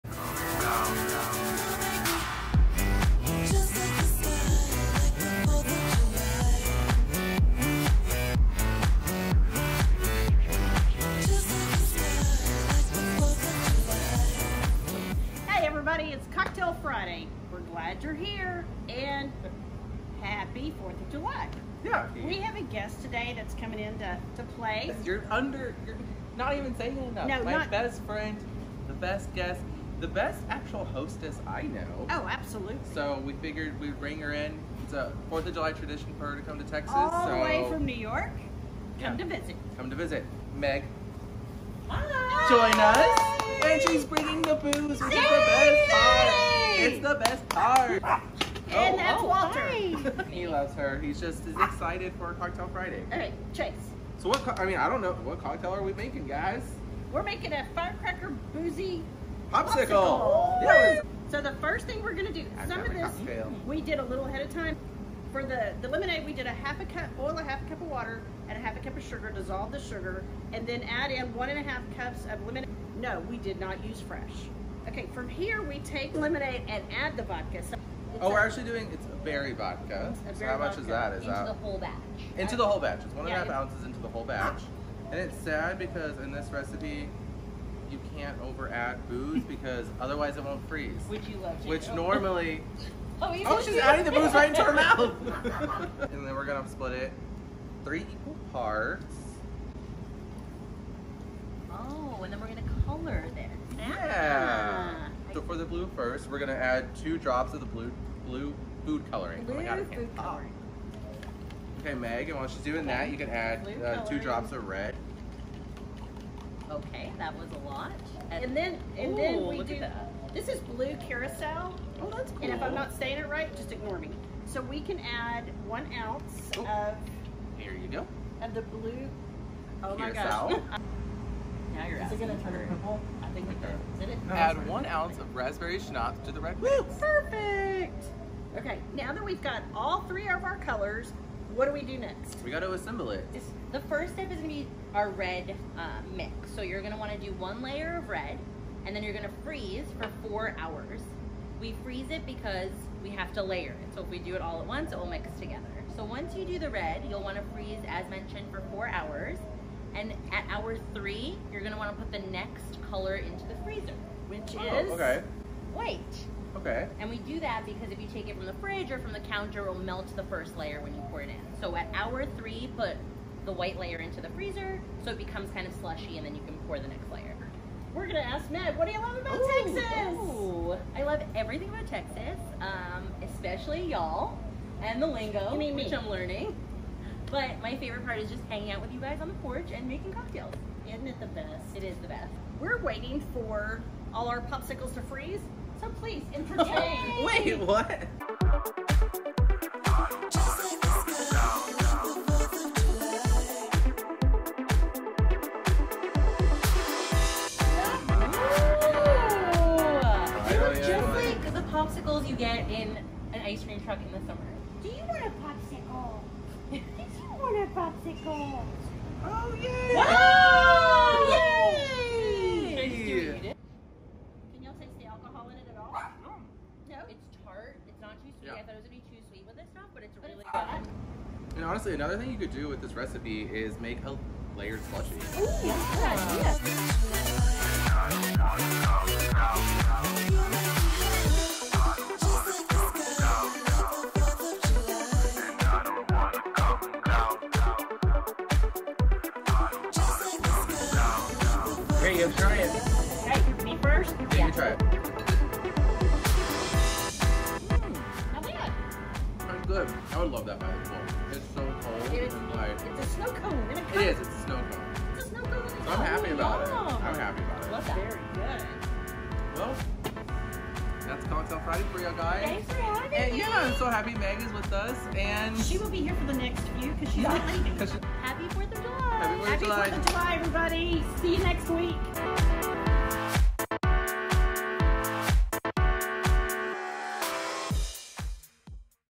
Cocktail Friday: Boozy Firecracker Popsicle
Instead of a cocktail we tried to make some boozy popsicles. Harder than we expected (and a bit messy!). Recipe also makes a great Red White and Blue layered slushy! Perfect for 4th of July.